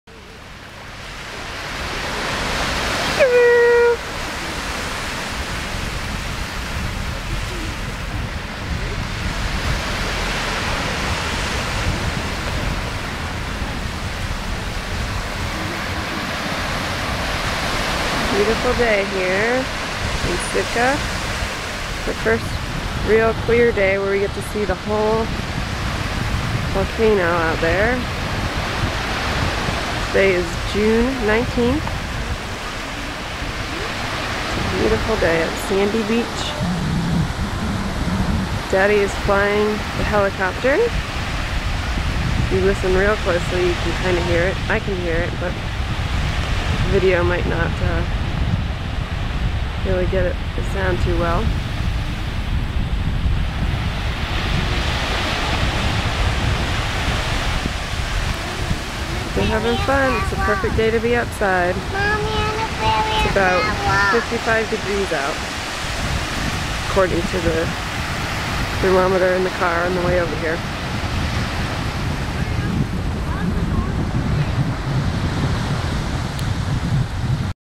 Sunny Day in Sitka
Sophia and Brett at sandy beach in Sitka, Mt Edgecombe in the background